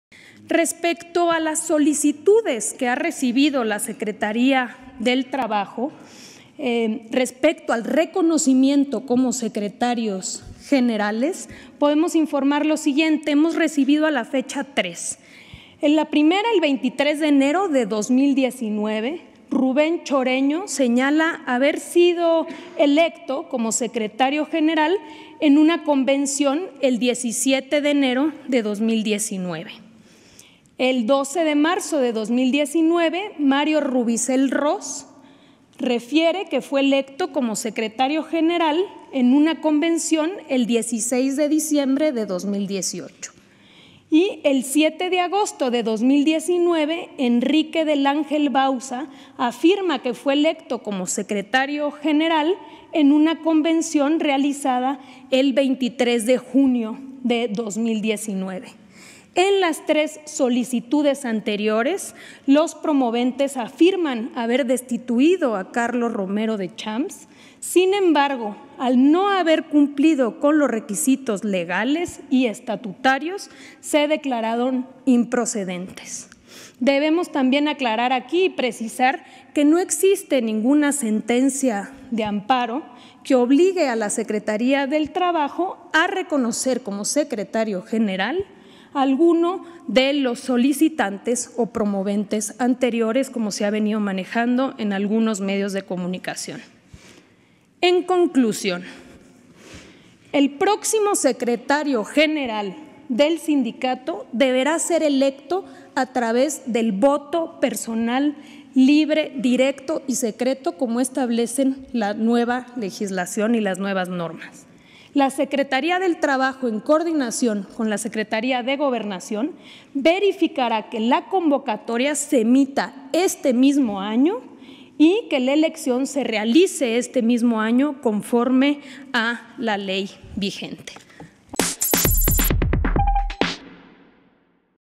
Hemos recibido 3 solicitudes de reconocimiento como Secretario General del STPRM: STPS
La titular de la Secretaría del Trabajo y Previsión Social, Luisa Alcalde, dijo «en conclusión el próximo secretario general del STPRM deberá ser electo a través del voto personal, libre, directo y secreto, como establece la nueva legislación y las nuevas normas».